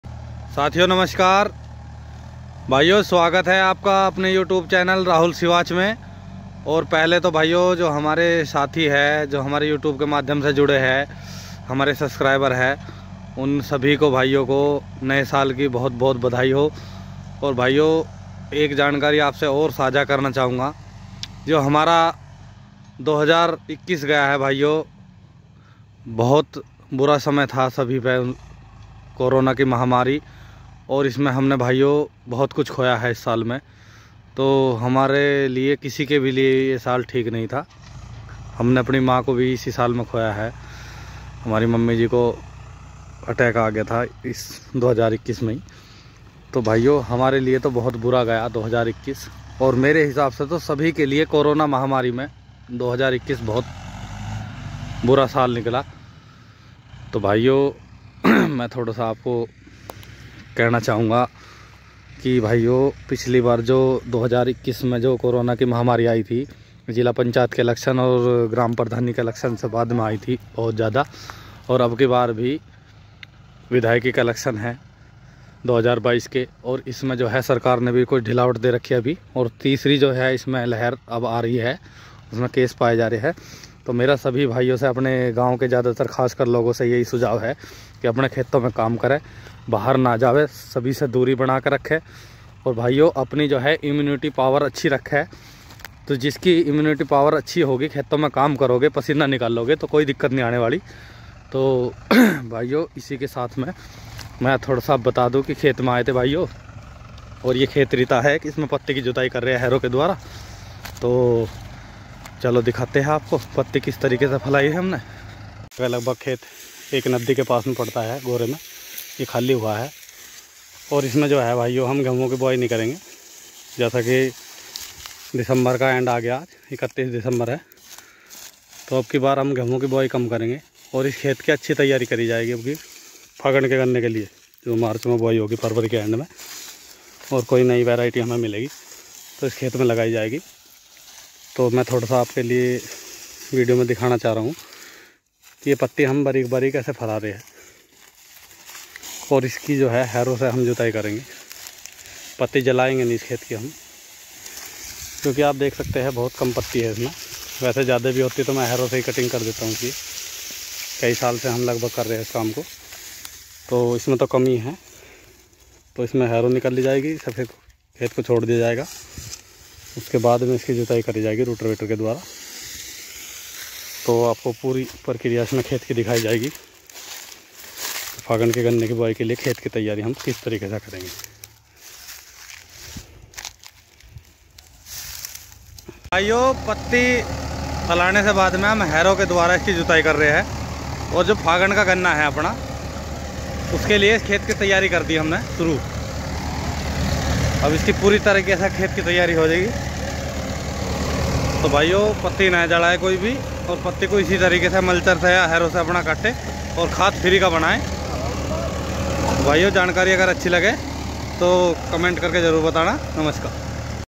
सभी भाइयों को 2022 की हार्दिक शुभकामनाएं फागन के गन्ने की बुवाई के लिए खेत की तैयारी शुरू
हैप्पी न्यू ईयर 2022#rahulsiwach# इस साल के लिए फागन के गन्ने के लिए खेत की तैयारी हमने कर दी शुरू हमारा खेत की तैयारी का तरीका